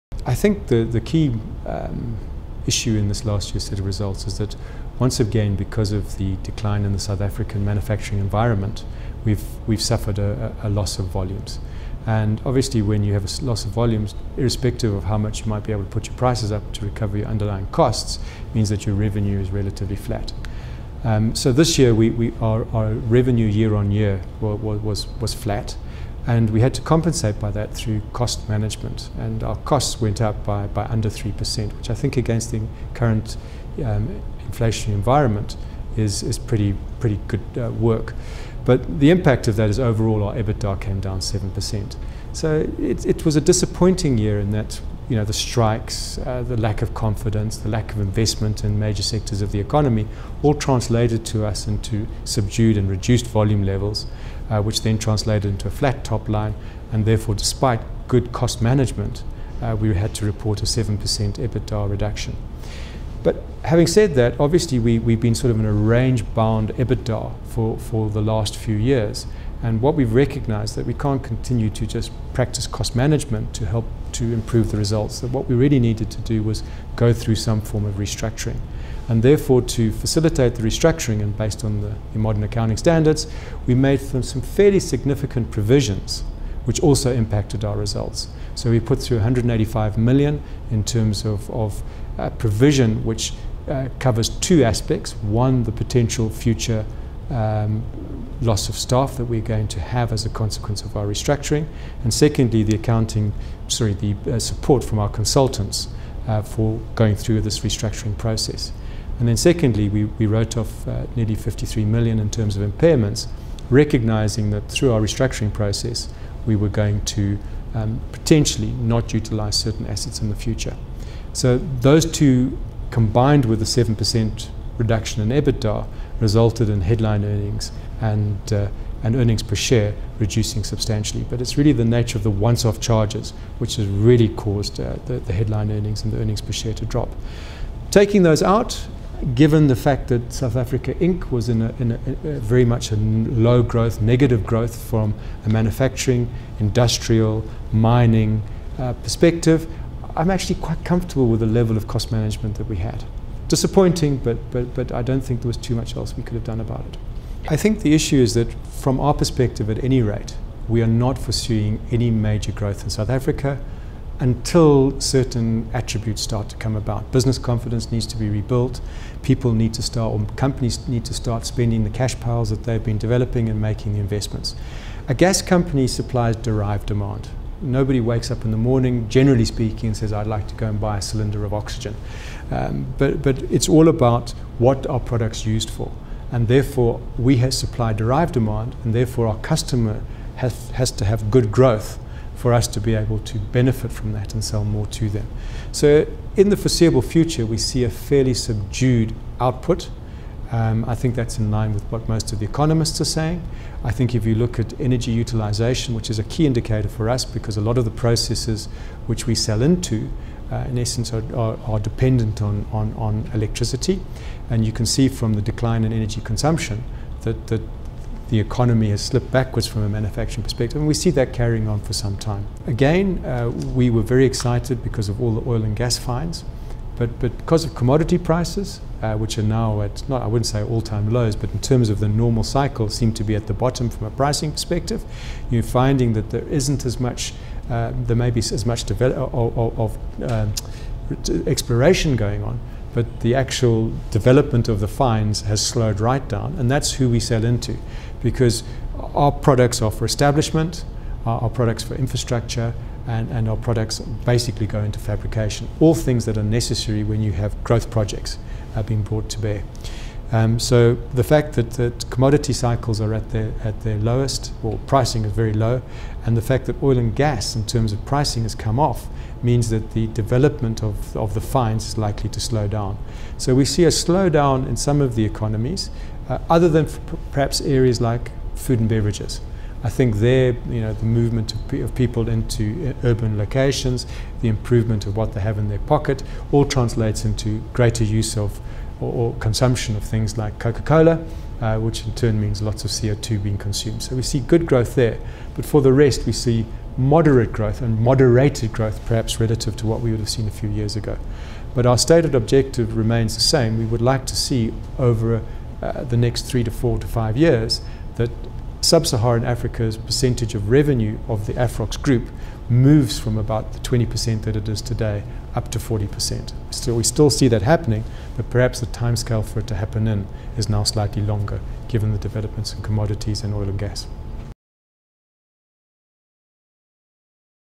Afrox expects job losses amid restructuring
JSE-listed African Oxygen (Afrox) expects a “reasonable number” of job losses to occur up to the third quarter of this year, as the company restructures its operations.